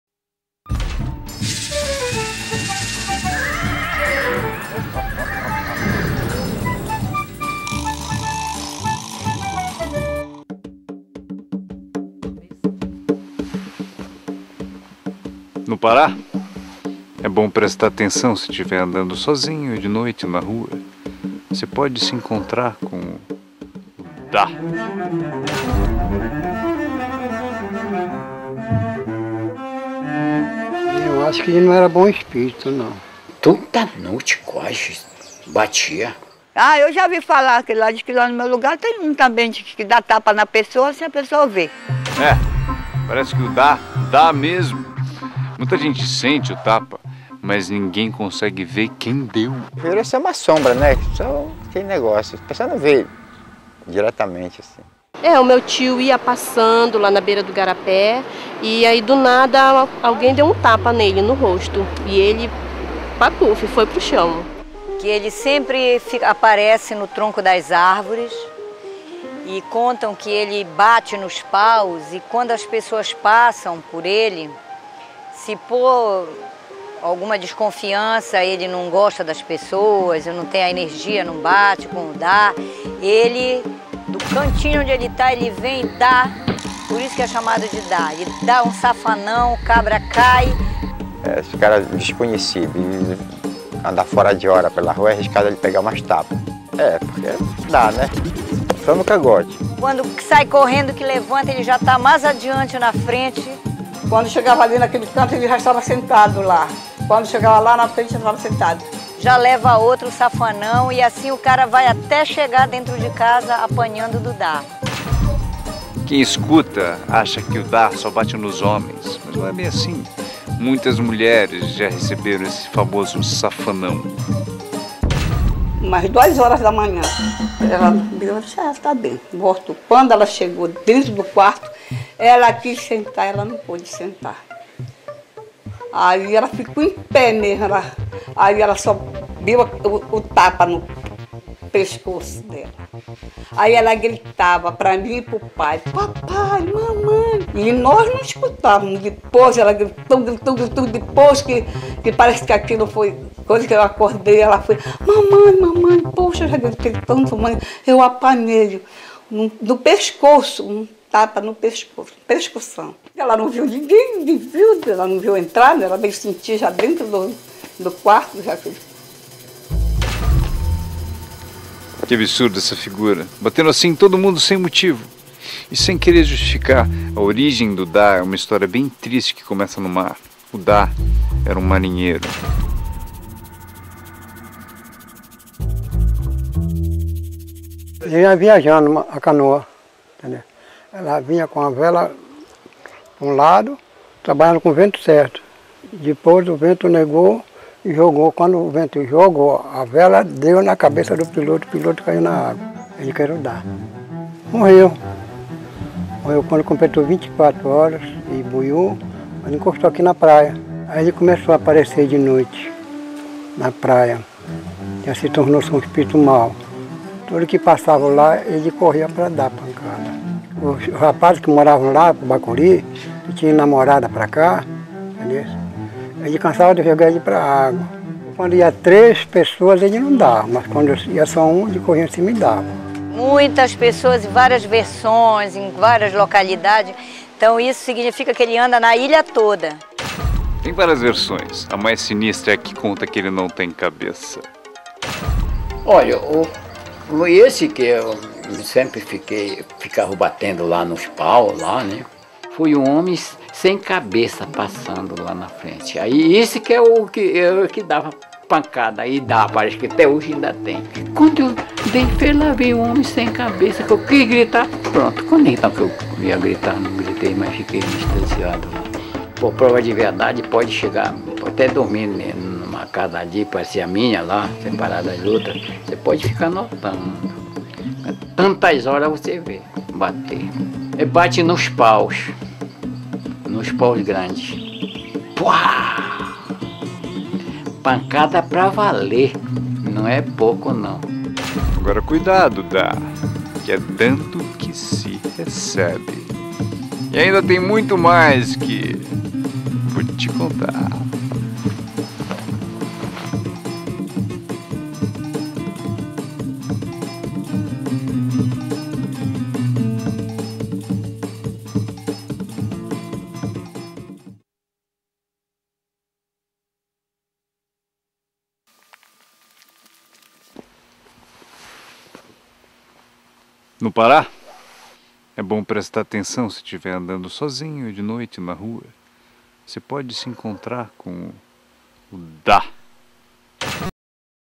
Vou te contar 2008 | Ep. 14: Dá
Vítima de naufrágio, sem sobreviver, vê-se preso em terras estrangeiras. Revoltado com essa situação, passa a atacar passantes em estradas desertas, dando tapas nos transeuntes, agredindo-os fisicamente. Em geral, ele não é visto. Mas em alguns casos é descrito como um homem muito alto, com ou sem cabeça, e outras como um menino, que mesmo assim dá pancadas.